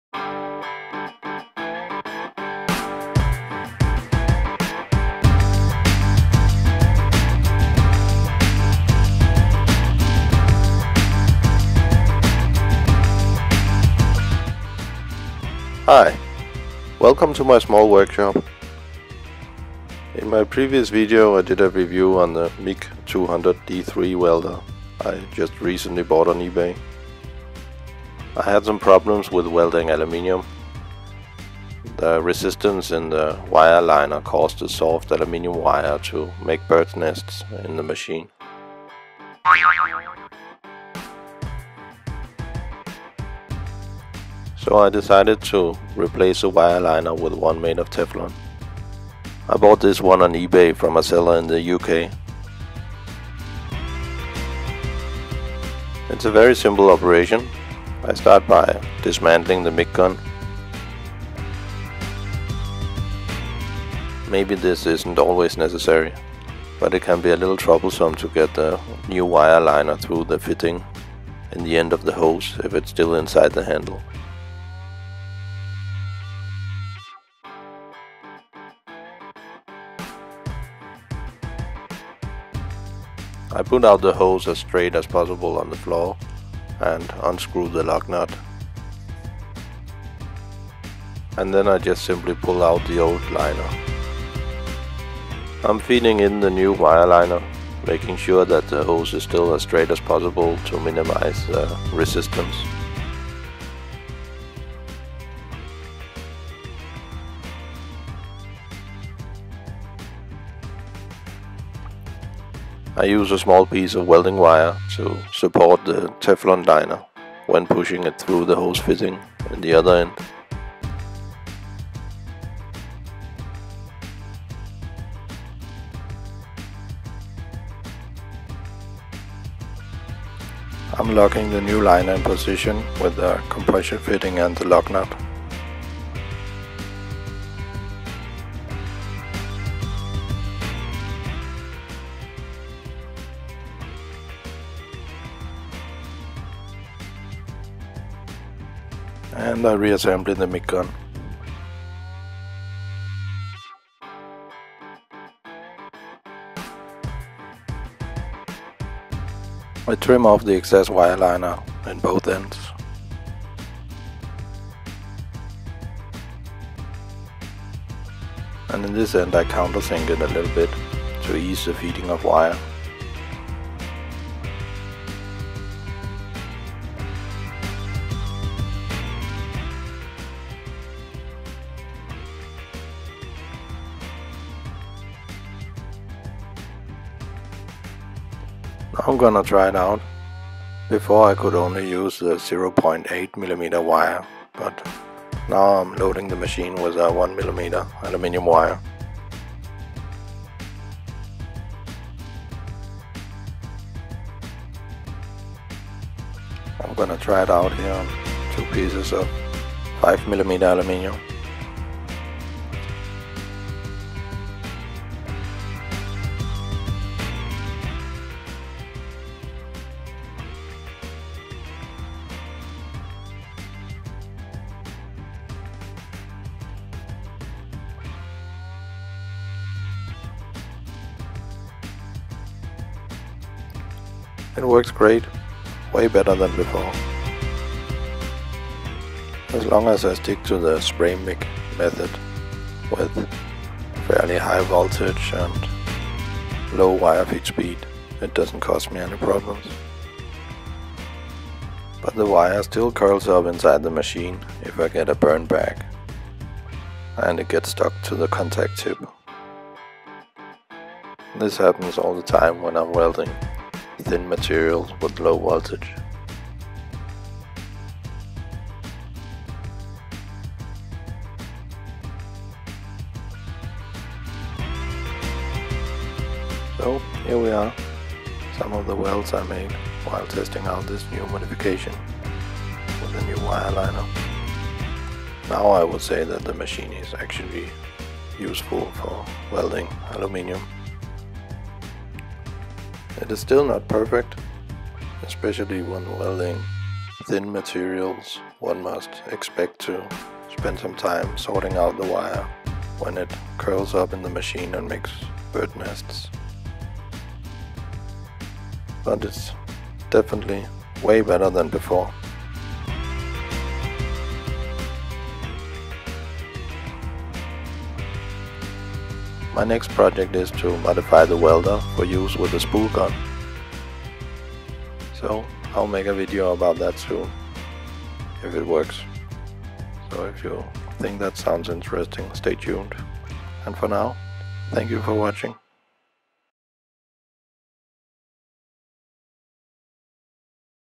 New teflon wire liner for the MIG200D3 mig gun - for improved aluminium welding.
I had some problems with the original wire liner when welding aluminium.  It was too tight for the 1 mm wire i normally use, so I had to use 0,8 mm. But even then the wire kept curling up in the machine (bird nesting) due to the resistance in the wire liner.
So I fitted a new teflon wire liner. It helped a lot.

I should mention that the original wire liner works just fine for welding with flux core and solid steel wire...